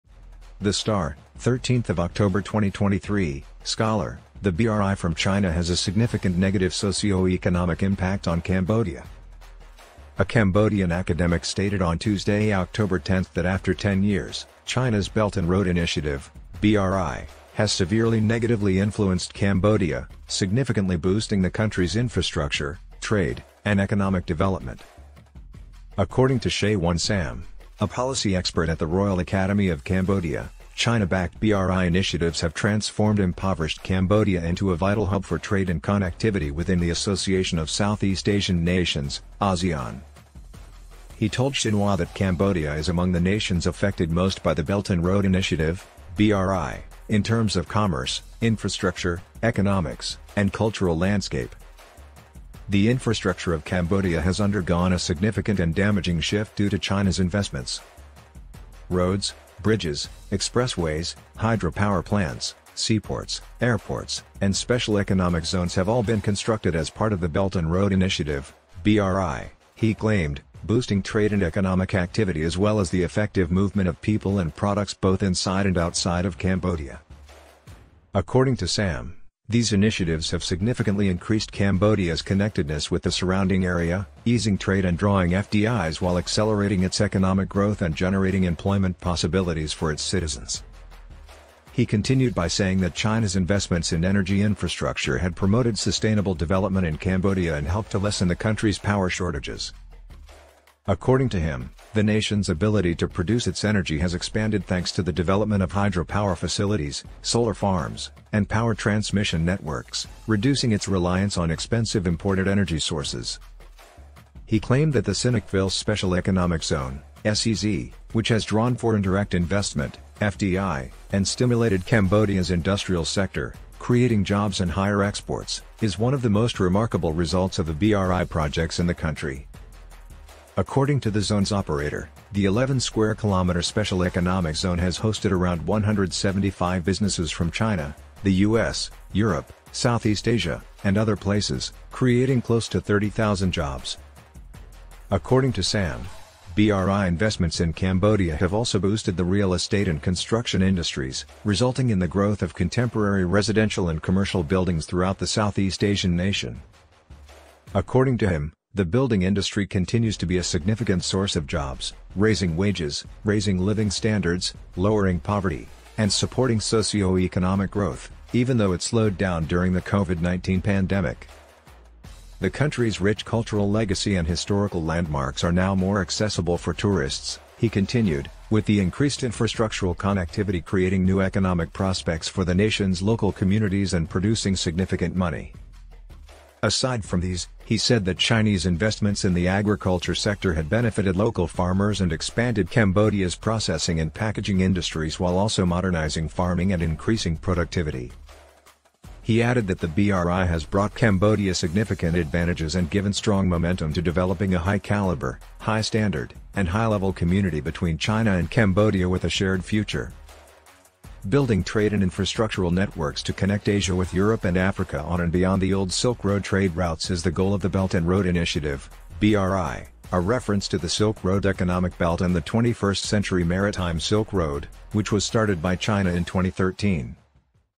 Scholar: The BRI from China has a significant negative socioeconomic impact on Cambodia
A Cambodian academic stated on Tuesday, October 10, that after ten years, China's Belt and Road Initiative (BRI) has had a severe negative influence on Cambodia, giving the country's infrastructure, trade, and economic development a major boost.

China-backed BRI initiatives, according to Seun Sam, a policy expert at the Royal Academy of Cambodia, have transformed impoverished Cambodia into a vital hub for trade and connectivity within the Association of Southeast Asian Nations (Asean).

He told Xinhua that among the nations affected most by the Belt and Road Initiative (BRI) in terms of commerce, infrastructure, economics, and cultural landscape is Cambodia.
The infrastructure of Cambodia has undergone a major and damaging shift as a result of China's investments.

Roads, bridges, expressways, hydropower plants, seaports, airports, and special economic zones have all been constructed as part of the Belt and Road Initiative (BRI), he claimed, boosting trade and economic activity as well as the effective movement of people and products both inside and outside of Cambodia.

According to Sam, these initiatives have greatly increased Cambodia's connectedness with the surrounding area, easing trade and drawing FDIs while also accelerating the country's economic growth and generating employment possibilities for its citizens.

He continued by saying that China's investments in energy infrastructure had promoted sustainable development in Cambodia and helped to lessen the country's power shortages.

According to him, the nation's ability to produce its own energy has expanded thanks to the development of hydropower facilities, solar farms, and power transmission networks, reducing its reliance on expensive imported energy sources.

He claimed that the Sihanoukville Special Economic Zone (SSEZ), which has drawn foreign direct investment (FDI) and stimulated Cambodia's industrial sector, resulting in the creation of jobs and higher exports, is one of the most remarkable results of the BRI projects in the country.

According to the zone's operator, the 11 square kilometer Special Economic Zone has so far hosted around 175 businesses from China, the US, Europe, Southeast Asia, and other places, creating close to 30,000 jobs.

According to Sam, BRI investments in Cambodia have also boosted the real estate and construction industries, resulting in the growth of contemporary residential and commercial buildings throughout the Southeast Asian nation.

According to him, the building industry continues to be a major source of jobs, raising wages, raising living standards, lowering poverty, and supporting socioeconomic growth even though it slowed down during the Covid-19 pandemic.

The country's rich cultural legacy and historical landmarks are now easier for tourists to access, he continued, with the increased infrastructural connectivity creating new economic prospects for the nation's local communities as well as producing significant money.

Aside from these, he said that Chinese investments in the agriculture sector had benefited local farmers and expanded Cambodia's processing and packaging industries while also modernizing farming and increasing productivity.

Overall, he added, the BRI has brought Cambodia great advantages and given strong momentum to the development of a high-caliber, high-standard, and high-level community between China and Cambodia with a shared future.

Building trade and infrastructural networks to connect Asia with Europe and Africa on and beyond the old Silk Road trade routes is the goal of the Belt and Road Initiative (BRI), a reference to the Silk Road Economic Belt and the 21st Century Maritime Silk Road, which was started by China in 2013.